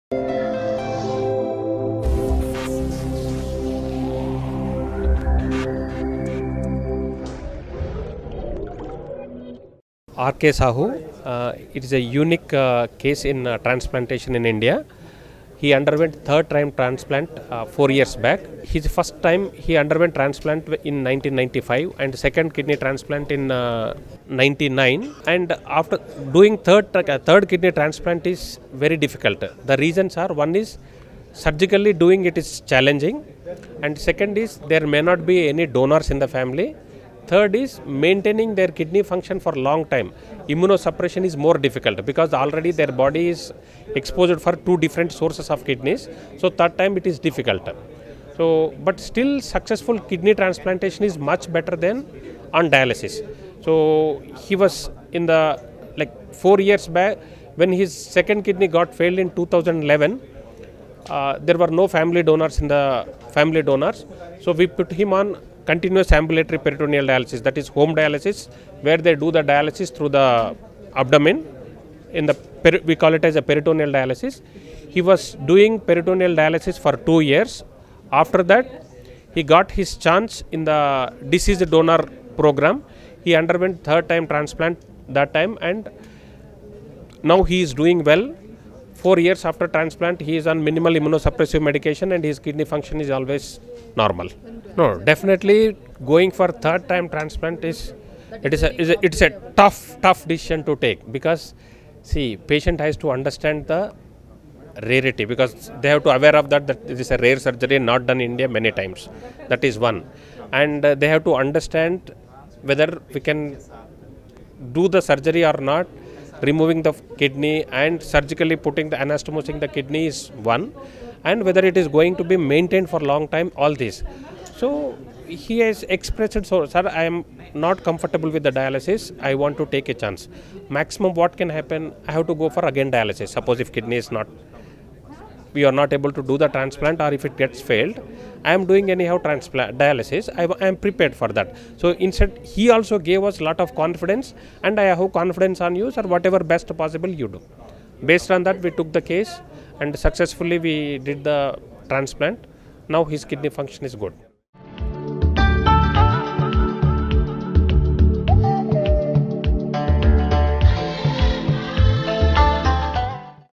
Sridhar | Successful Kidney Transplantation is better than on dialysis| hybiz
G Sridhar MD, Nephrologist Gleneagles Global Hospitals . Successful Kidney Transplantation is better than on dialysis